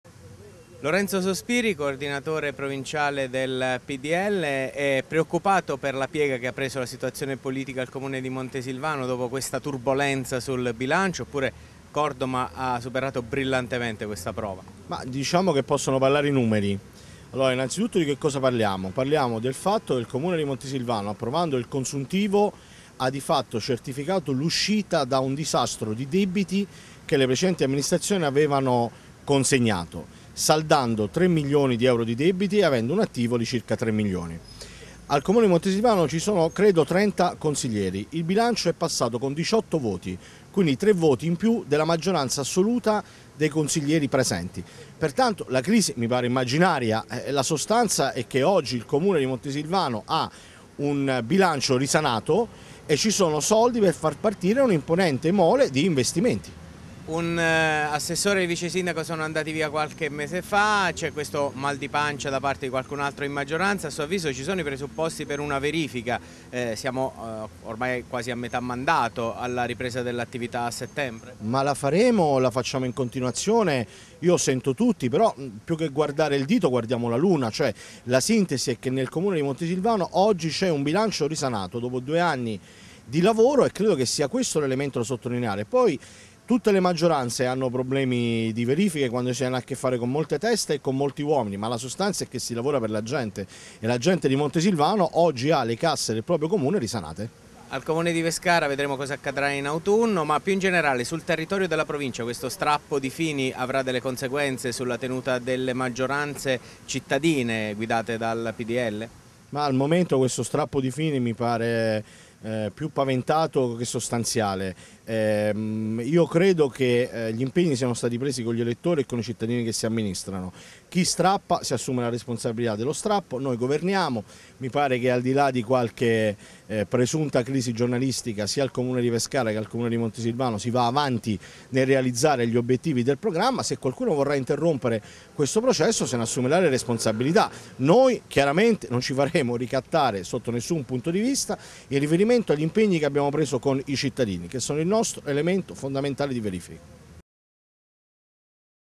"Maggioranza solida per Cordoma"
Dopo le tensioni sul bilancio a Montesilvano al Tgsei parla il coordinatore provinciale del PDL Sospiri: maggioranza solida per Cordoma, ma non ci sottraiamo ad una eventuale verifica in autunno. Nelle amministrazioni di centrodestra sul territorio lo strappo di Fini -afferma- non avrà ripercussioni